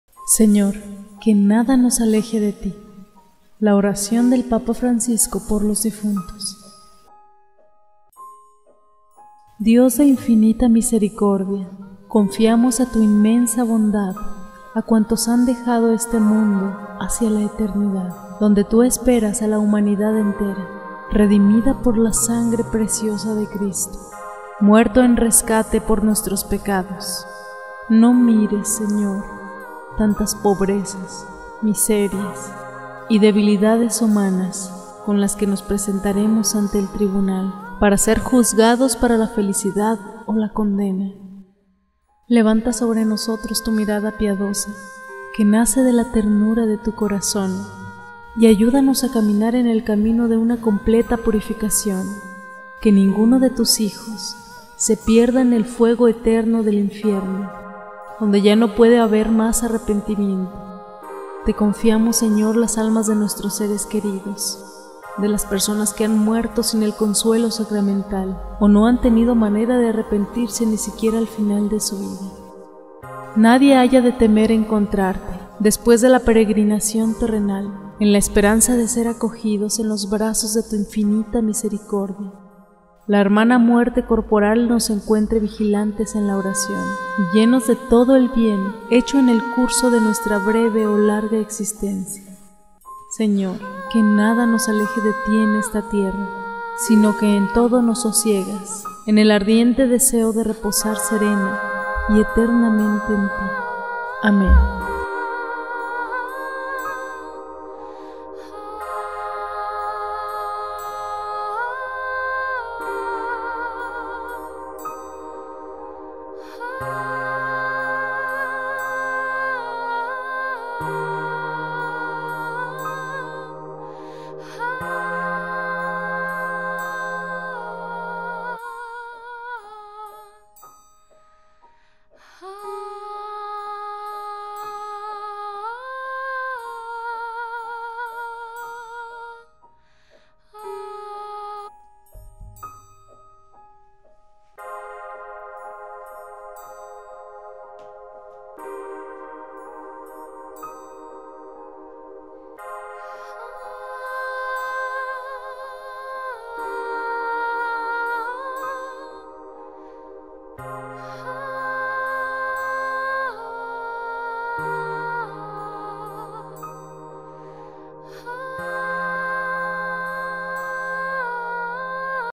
ORACIÓN POR LOS DIFUNTOS
Señor que nada nos aleje de tí, la oración del papa Francisco por los difuntos